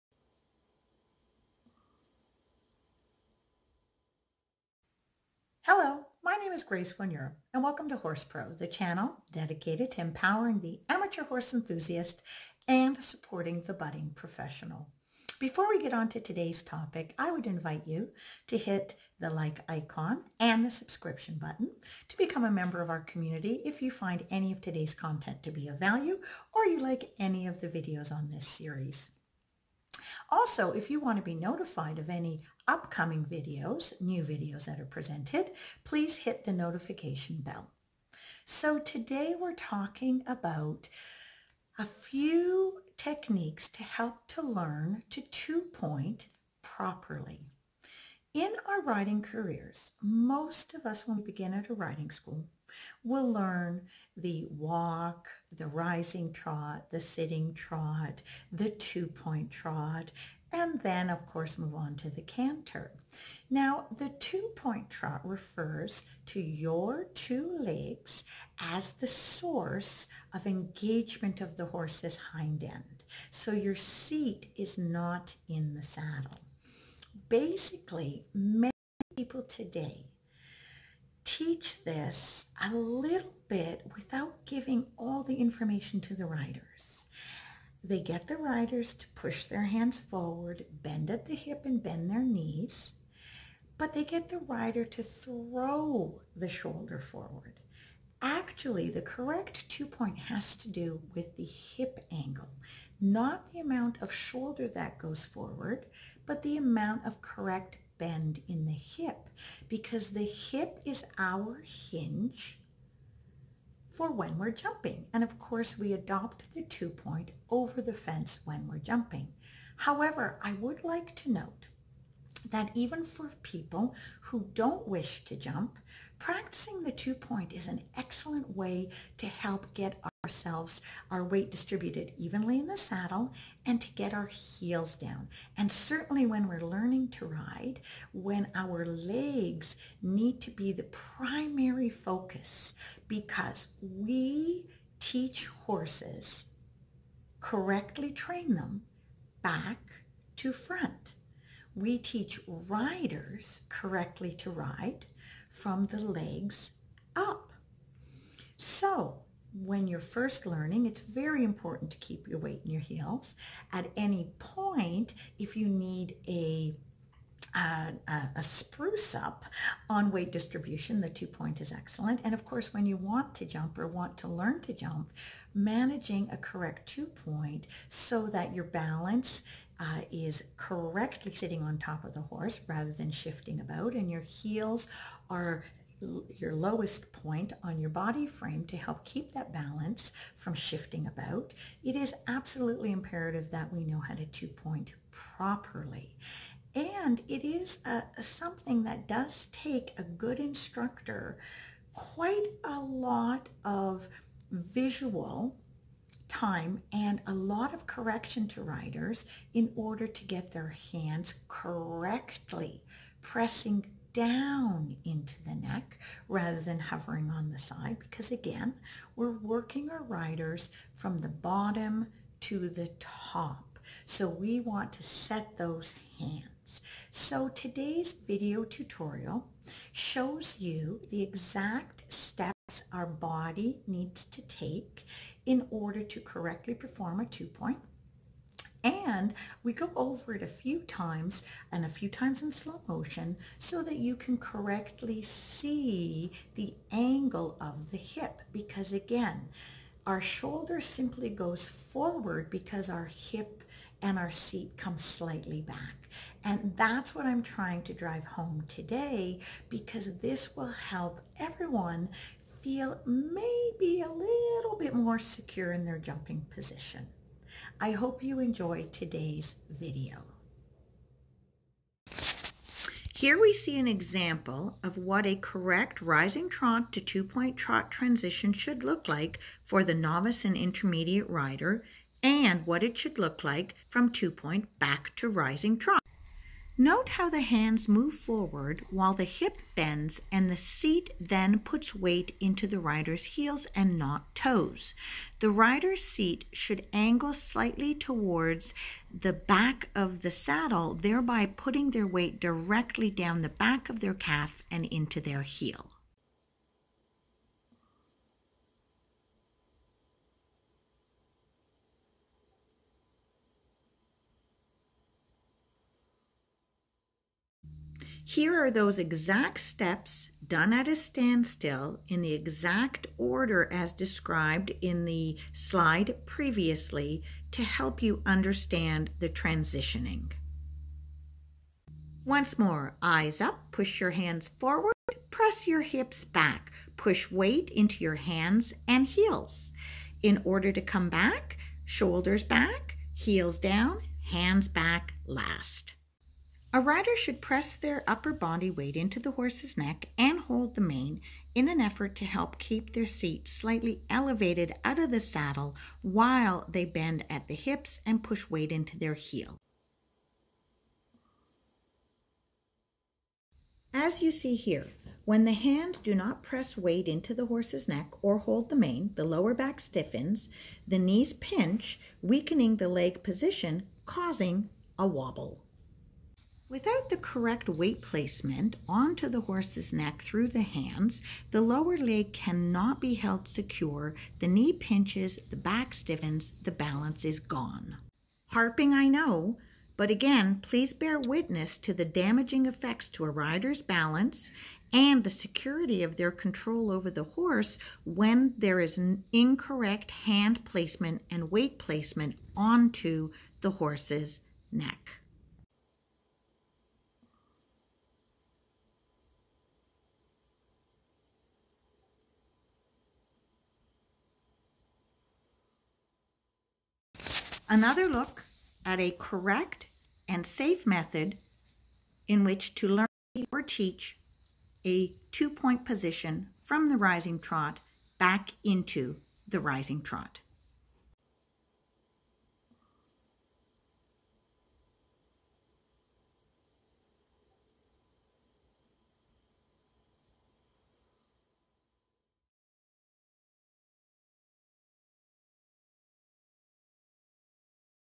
Mastering the 2 Point position to improve your balance and leg position! How to demo included!
Video demo tutorial begins at min 4:31.

Whether a rider wishes to learn to jump, or not, a properly taught 2 point is an easy and safe way to teach someone a correct leg position, one in which the weight is in their heels, providing stability and balance.
      Stability of the lower legs allows a rider to guide the horse more positively, with confidence, and allows for their skill set to multiply.
      Balance is a key ingredient to riding a horse safely and properly so that both horse and rider remain confident and happy.
       Today I am thankful to Maryann Chilton for providing the demo riding, on Ingleside Farm's horse, Noemi. Thank you to the Esdale family for the use of their mare and their private facility.

Follow me @horsepro.ca.

FB: HorsePro or Rocabarra Farm.